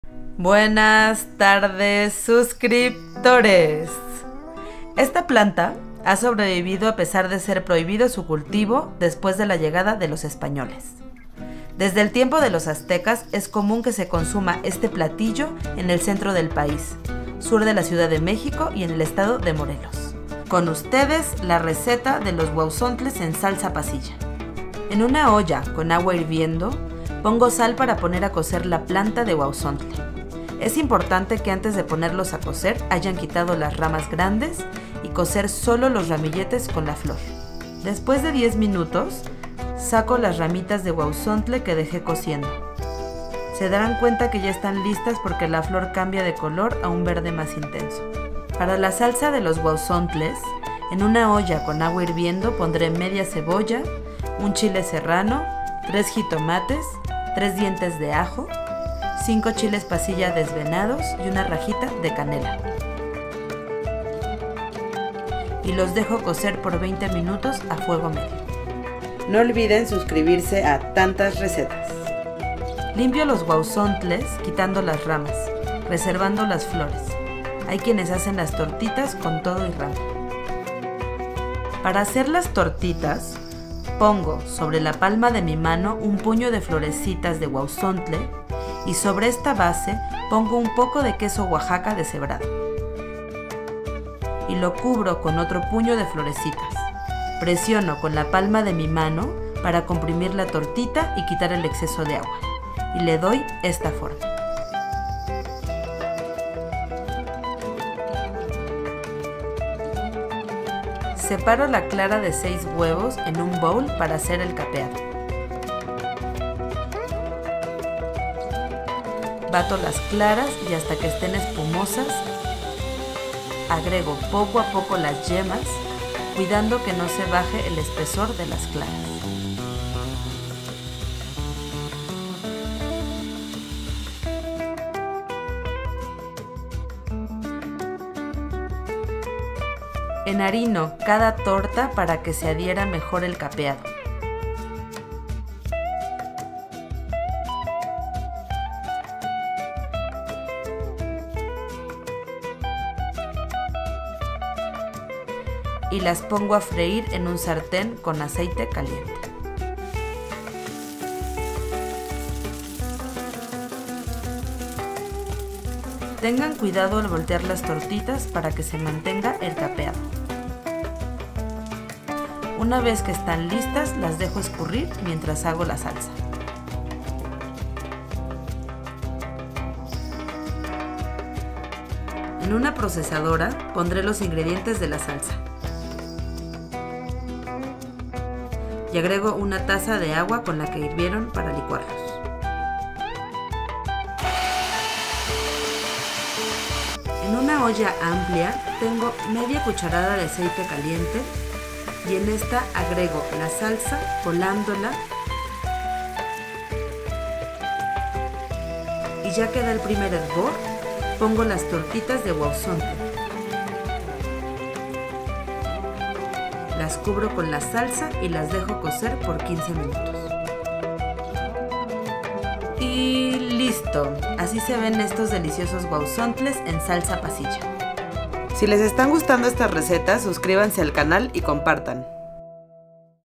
Huauzontles en Salsa Pasilla - Tan Tas Recetas
Desde el tiempo de los aztecas es común que se consuma este platillo en el centro del país, sur de la ciudad de México y en el estado de Morelos. Con ustedes la receta de los Huauzontles en Salsa Pasilla.